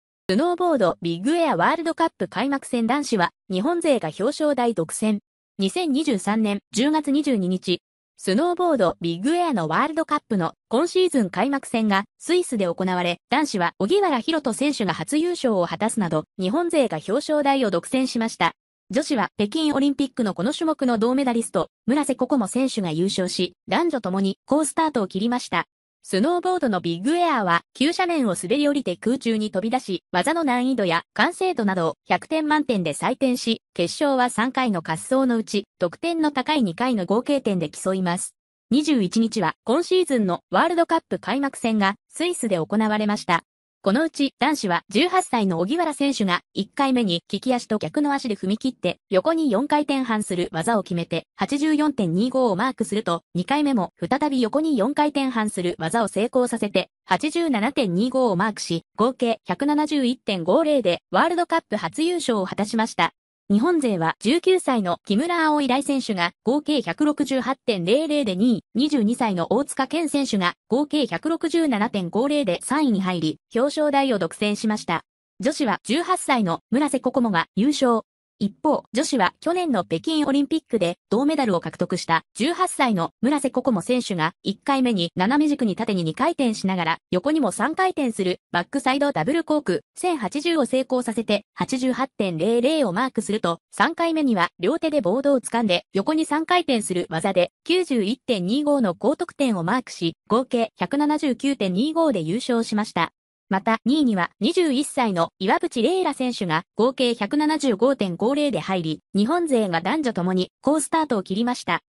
スノーボードビッグエア W杯開幕戦 男子は日本勢が表彰台独占
2023年10月22日

スノーボードビッグエアのワールドカップの今シーズン開幕戦がスイスで行われ、男子は荻原大翔選手が初優勝を果たすなど日本勢が表彰台を独占しました。女子は北京オリンピックのこの種目の銅メダリスト、村瀬心椛選手が優勝し、男女ともに好スタートを切りました。

スノーボードのビッグエアは急斜面を滑り降りて空中に飛び出し、技の難易度や完成度などを100点満点で採点し、決勝は3回の滑走のうち、得点の高い2回の合計点で競います。